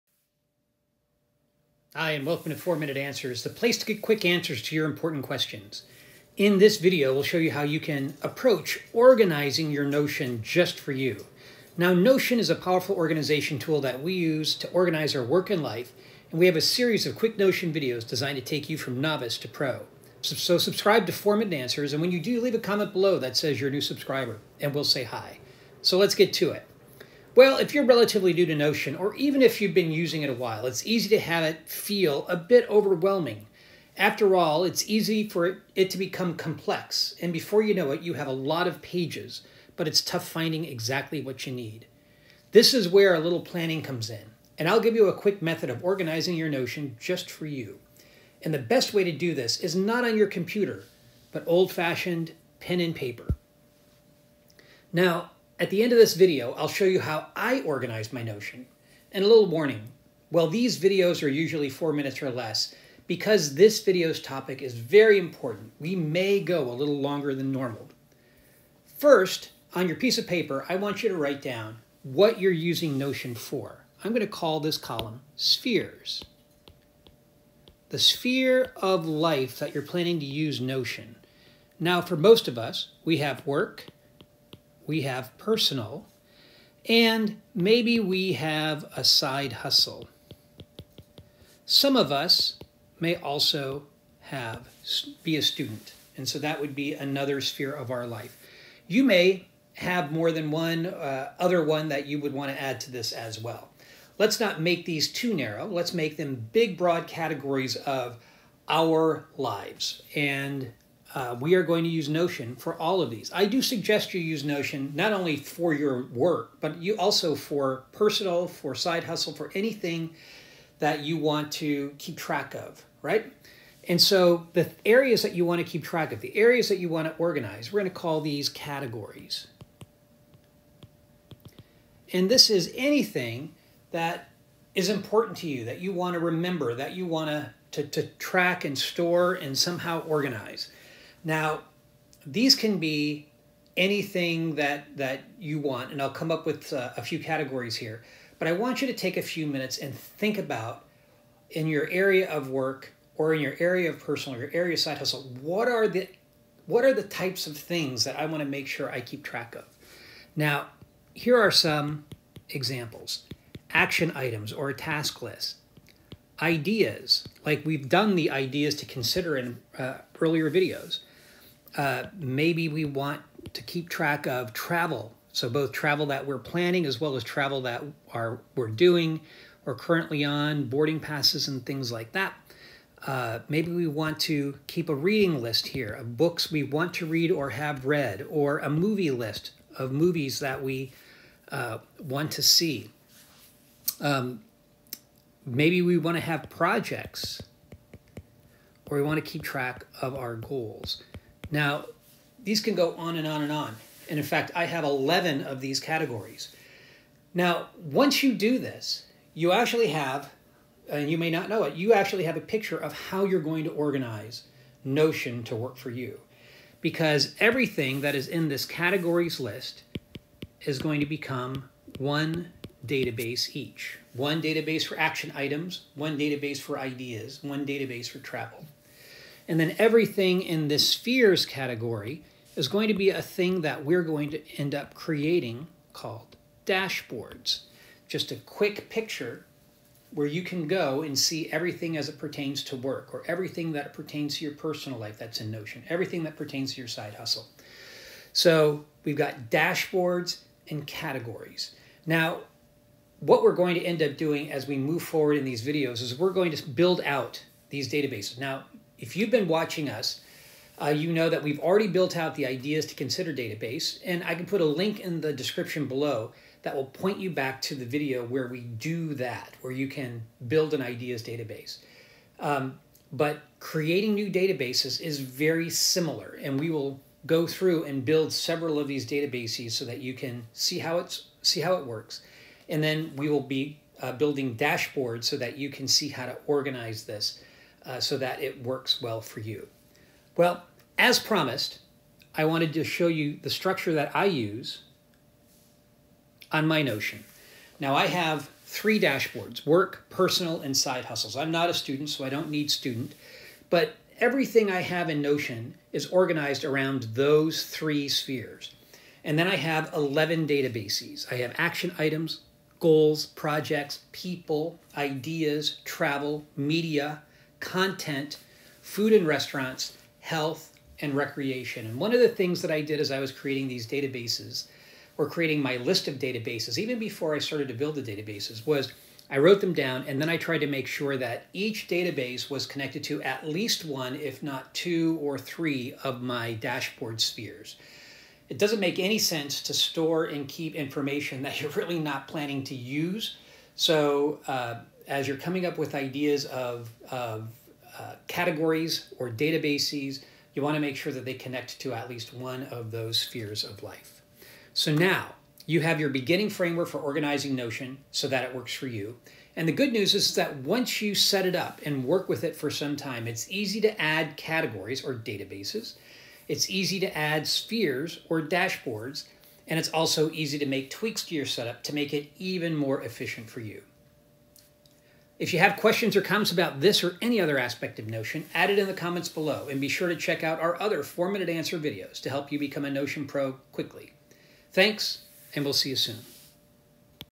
Organizing Notion to Work for You | Notion Tutorial
In this Notion tutorial video, you will learn how to organize Notion to work for you.  Unlike our other videos, this one is a little longer than 4 minutes, and it is because this topic is so important we wanted to make sure we covered it thoroughly. You'll also see how we have set up Notion for our own use.

Prior videos that you may want to reference are:

Types of Databases in Notion (coming soon)